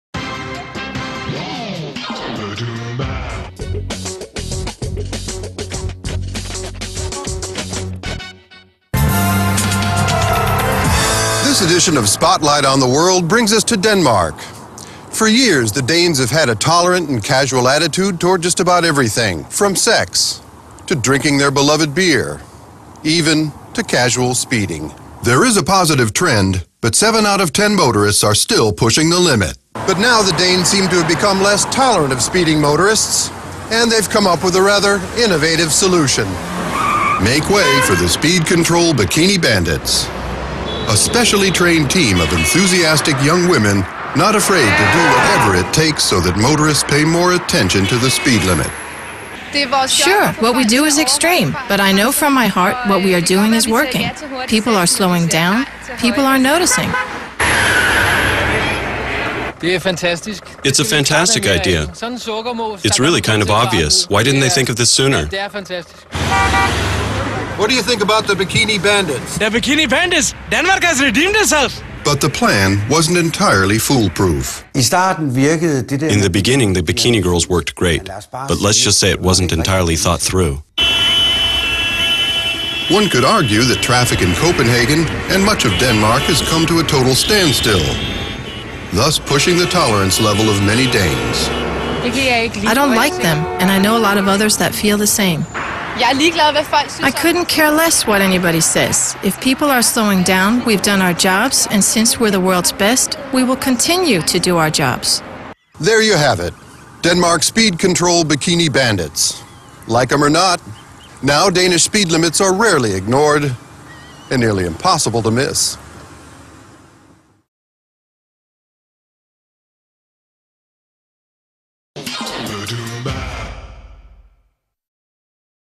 Traffic control in Denmark with the bikini bandits
The bikini bandits help reduce speeding on Danish roads